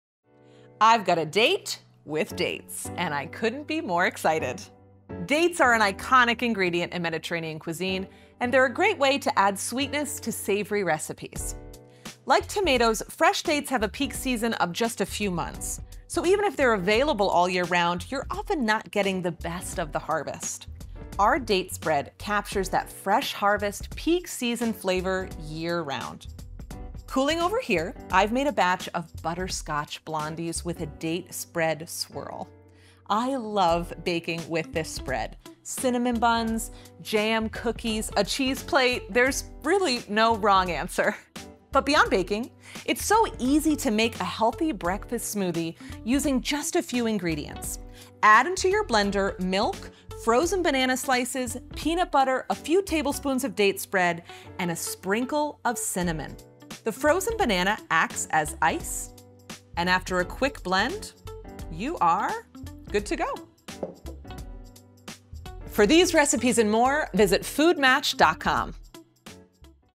The Dish with Divina | Date Spread
About Divina Date Spread

This is one date that's surely going to turn into a love story! Our Date Spread is handcrafted from sundried dates (and not much else!) and boasts a deeply fruit-forward flavor profile. We love it on cheese boards, spread over toast with almond butter or blended into a smoothie.

Having made many 'top trends of the year' lists, dates are growing in popularity for their wonderful flavor and health benefits. This is a truly versatile spread that pairs well with sweet and savory dishes.

Case pack:
12/9 oz Glass Jar